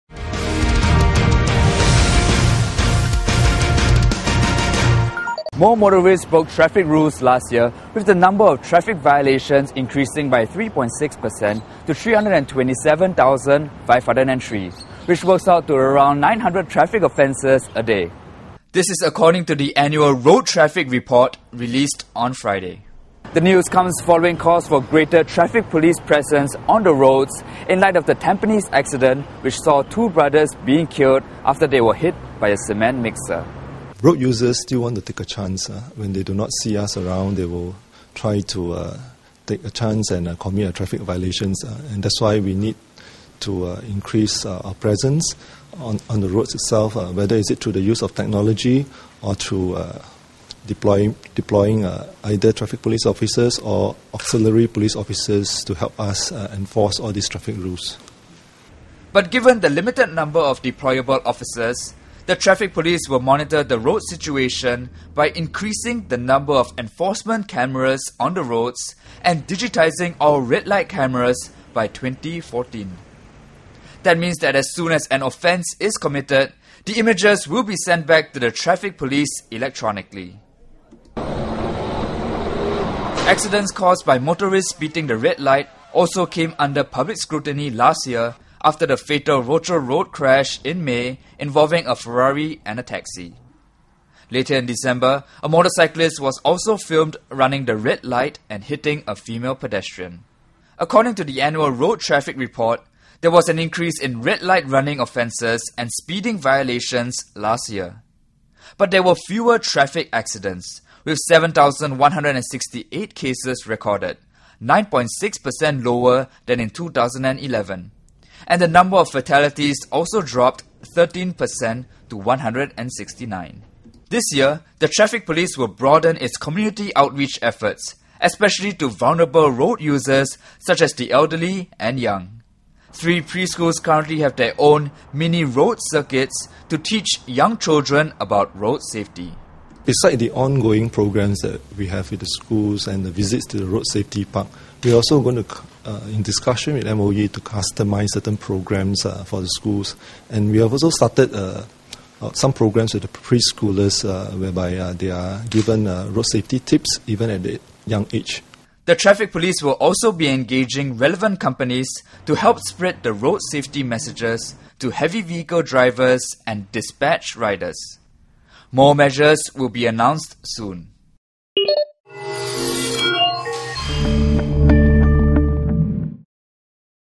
900 traffic offences a day
Find out how the traffic police will be addressing the increase in traffic violations last year and which offences are on the rise.